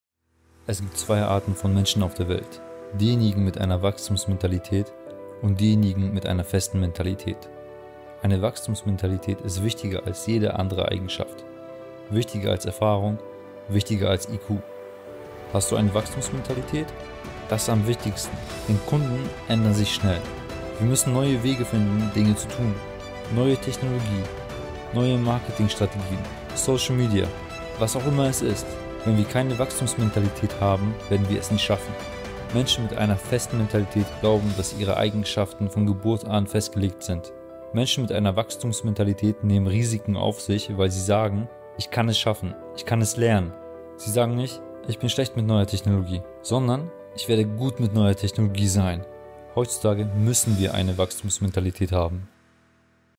Hast du eine Wachstumsmentalität?!?
Kurzer Auszug aus den Schriiften von Carol Dweck.
Carol Susan Dweck ist eine amerikanische Professorin für Psychologie an der Stanford University. Dweck ist bekannt für ihre Arbeit zu Motivation und Denkweise.

Foto von Patrick Hendry auf Unsplash